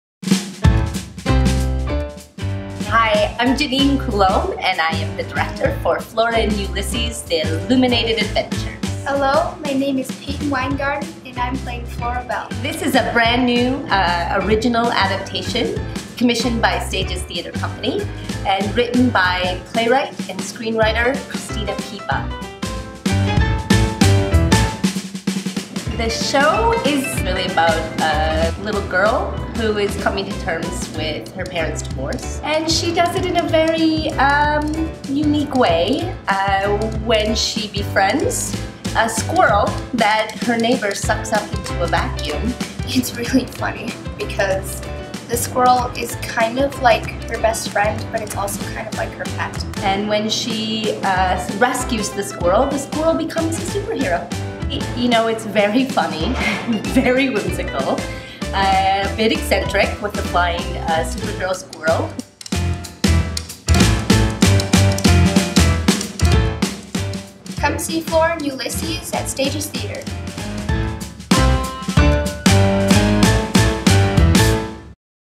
Introducing Flora & Ulysses at Stages Theatre Company
Flora & Ulysses: The Illuminated Adventures
Apr 28 – May 20, 2017  •  World Premiere Exclusive!

Script adaptation written by Cristina Pippa
Based on the book by Kate DiCamillo

The super-hero squirrel, Ulysses and the self-proclaimed cynic Flora Belle Buckman land in our Hopkins Jaycees Studio this spring in an inspiring story of being born anew. When Flora saves Ulysses after he is sucked up through a vacuum cleaner, neither could predict the changes that will ensue. This eccentric, endearing saga about the possibilities of hope and the promises of a capacious heart is a world premiere adaptation based on the #1 New York Times best-selling author, Kate DiCamillo’s 2014 Newbery Medal winner.

PLAY  |  RUNNING TIME: Approximately 75 minutes without an intermission.

ACCESSIBLE PERFORMANCES

Sunday, April 30 at 2 pm – Pay What You Can performance
Saturday, May 13 at 10 am – Sensory-Friendly performance